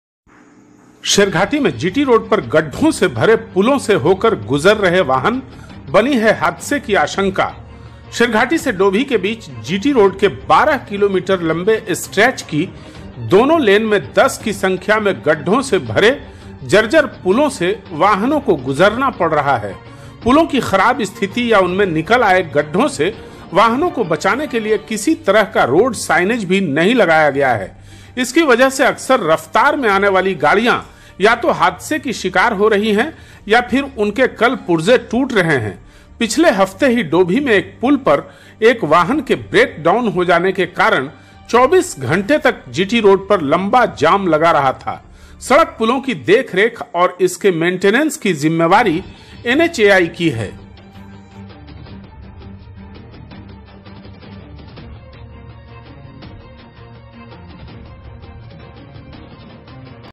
शेरघाटी में जीटी रोड पर गड्ढों वाले पुल से होकर गुजर रहे वाहन, हादसे की आशंका
शेरघाटी में जीटी रोड पर गड्ढों वाले पुल से होकर गुजर रहे वाहन, हादसे की आशंका शेरघाटी GT Road